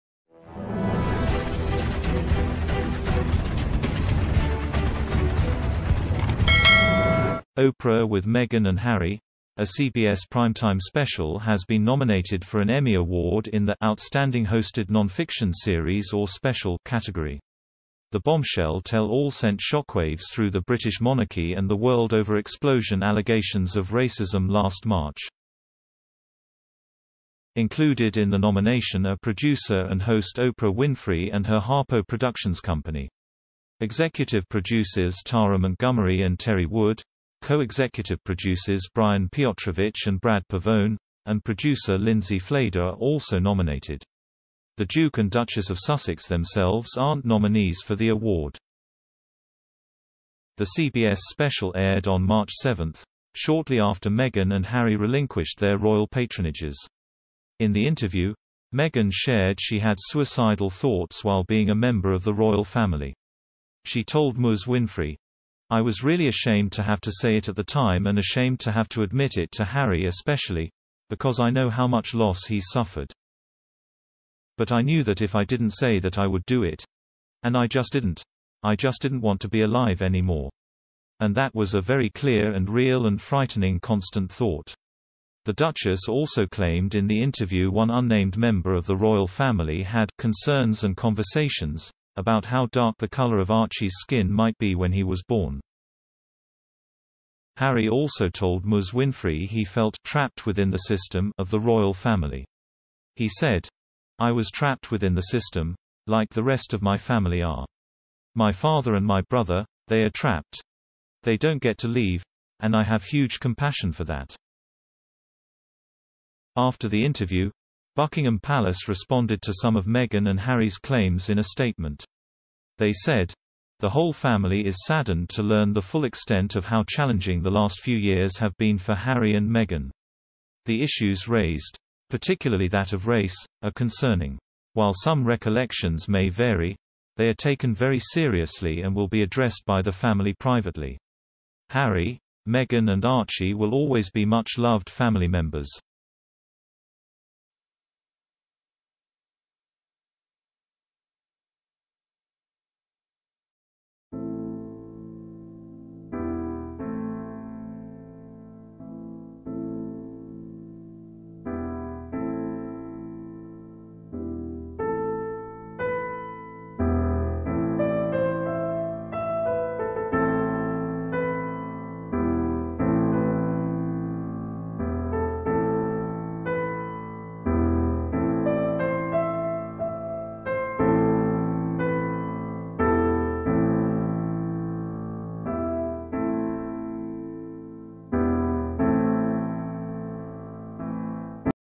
Meghan Markle and Prince Harry's bombshell Oprah interview nominated for major award
"Oprah with Meghan & Harry: A CBS Primetime Special" has been nominated for an Emmy Award in the “Outstanding Hosted Nonfiction Series or Special” category.

The bombshell tell-all sent shockwaves through the British monarchy and the world over explosion allegations of racism last March.

Included in the nomination are producer and host Oprah Winfrey and her Harpo Productions company.

Executive producers Tara Montgomery and Terry Wood, co-executive producers Brian Piotrowicz and Brad Pavone, and producer Lindsay Flader are also nominated.

The Duke and Duchess of Sussex themselves aren’t nominees for the award.

The CBS special aired on March 7, shortly after Meghan and Harry relinquished their royal patronages.